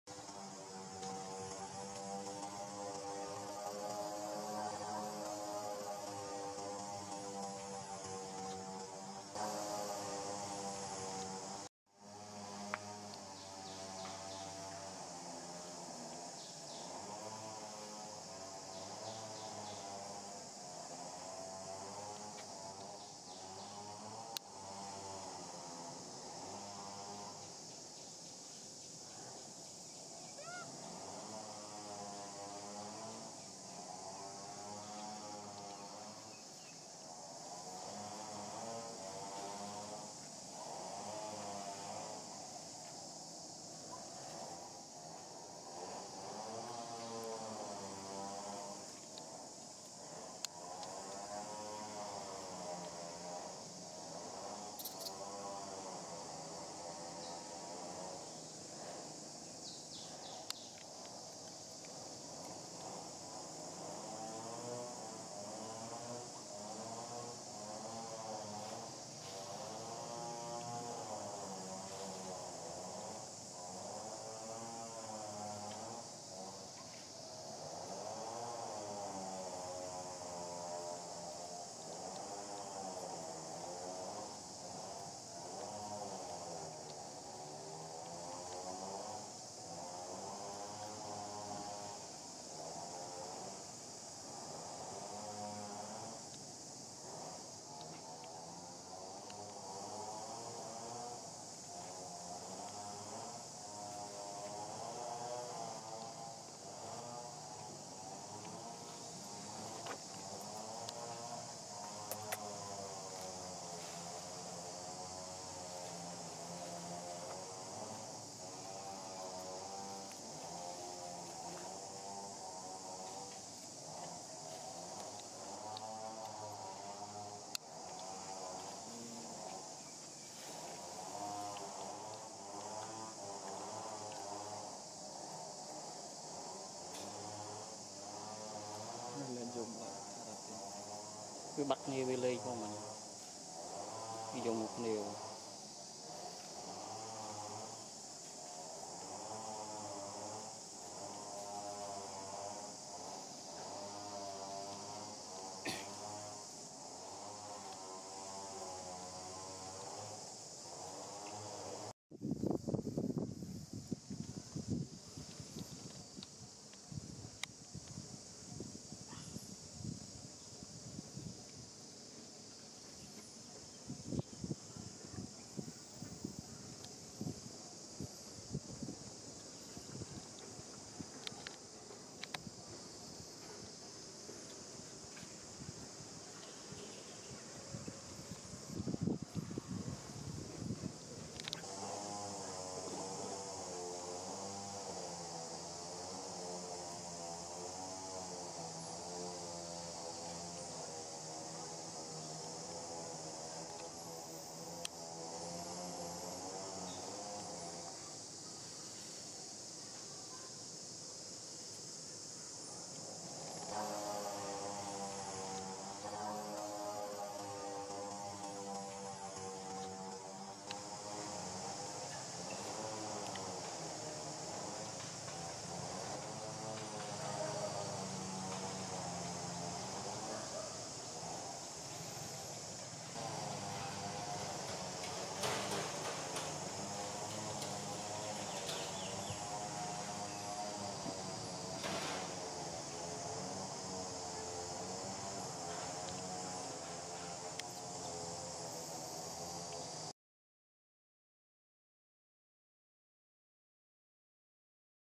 Welcome to Newborn Baby! Mother Rana Giving Her Newborn Baby and Baby Look So Cute
In this video, I would like to share you the story of Welcome to Newborn Baby! Mother Rana Giving Her Newborn Baby and Baby Look So Cute.
Please enjoy happily with this video monkey.